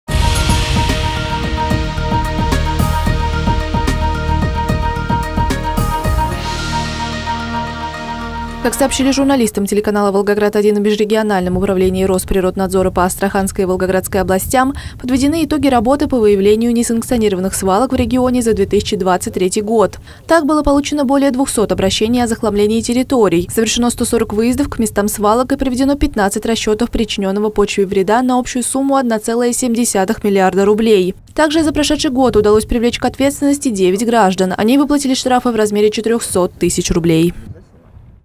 В 2023 году свалки в Волгограде и области нанесли ущерб природе на 1,7 млрд рублей
Как сообщили журналистам телеканала «Волгоград1» в Межрегиональном управлении Росприроднадзора по Астраханской и Волгоградской областям, подведены итоги работы по выявлению несанкционированных свалок в регионе за 2023 год. Так, было получено более 200 обращений о захламлении территорий, совершено 140 выездов к местам свалок и проведено 15 расчетов причиненного почве вреда на общую сумму 1,7 млрд рублей. Также за прошедший год удалось привлечь к ответственности 9 граждан, они выплатили штрафы в размере 400 тысяч рублей.